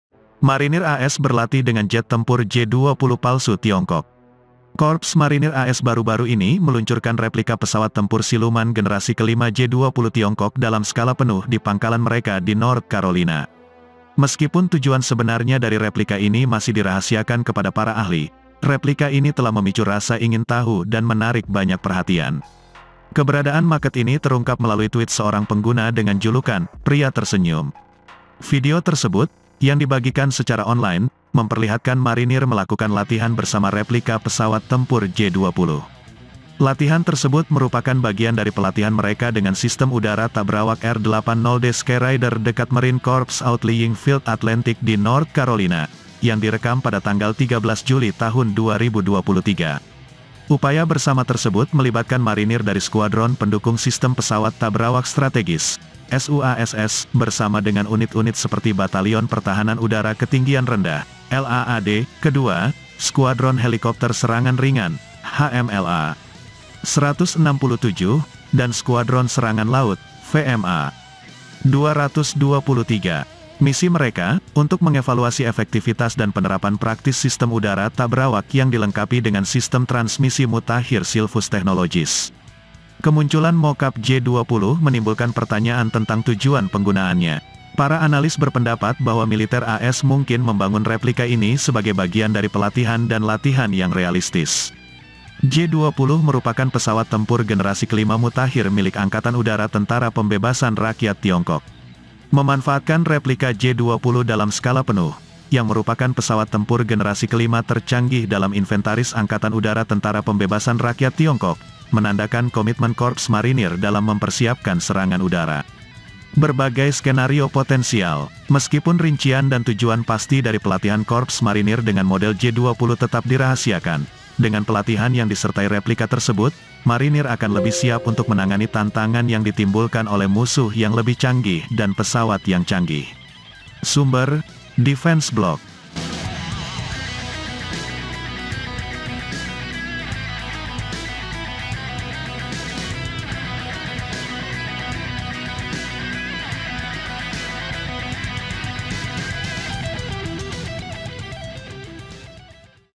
J-20 Replika Berlatih Bersama Marinir AS di North Carolina
Marinir AS berlatih dengan jet tempur J-20 palsu Tiongkok.
Korps Marinir AS baru-baru ini meluncurkan replika pesawat tempur siluman generasi kelima J-20 Tiongkok dalam skala penuh di pangkalan mereka di North Carolina. Meskipun tujuan sebenarnya dari replika ini masih dirahasiakan kepada para ahli, replika ini telah memicu rasa ingin tahu dan menarik banyak perhatian.
Keberadaan maket ini terungkap melalui tweet seorang pengguna dengan julukan (Pria Tersenyum). Video tersebut, yang dibagikan secara online, memperlihatkan Marinir melakukan latihan bersama replika pesawat tempur J-20. Latihan tersebut merupakan bagian dari pelatihan mereka dengan Sistem Udara Tak Berawak R80D SkyRaider dekat Marine Corps Outlying Field Atlantic di North Carolina, yang direkam pada 13 Juli 2023.

untuk selengkapnya silahkan ikuti
sumber: